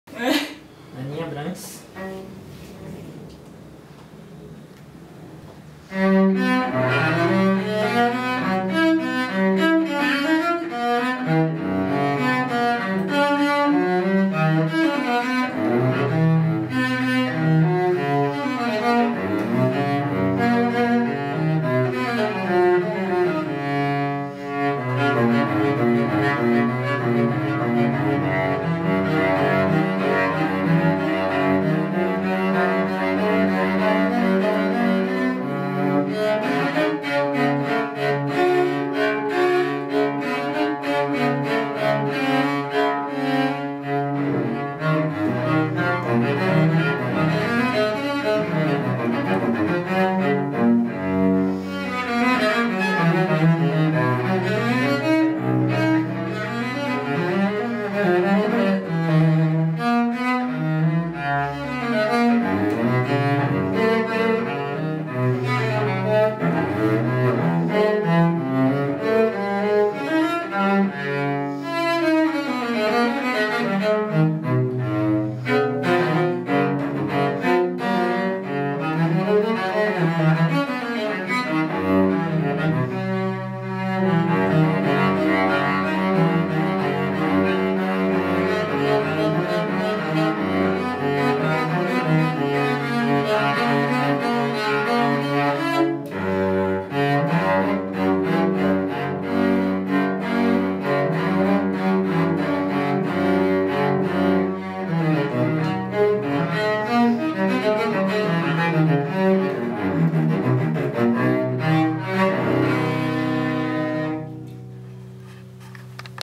Alessandro Gagliano violoncello
Violoncelista Ana Abrates testa seu novo violoncelo, copia e Alessandro Gagliano construido 1704 por Ivan Labussiere em 2011